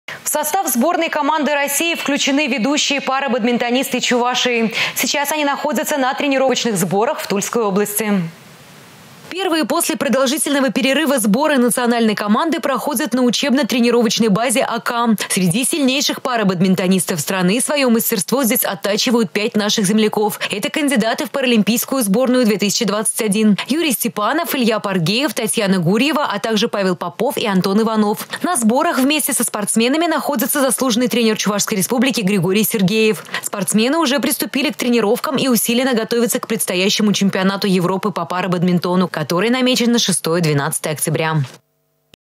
В состав сборной команды России включены ведущие парабадминтонисты Чувашии
В состав сборной команды России включены ведущие парабадминтонисты Чувашии. Сейчас они находятся на тренировочных сборах в Тульской области.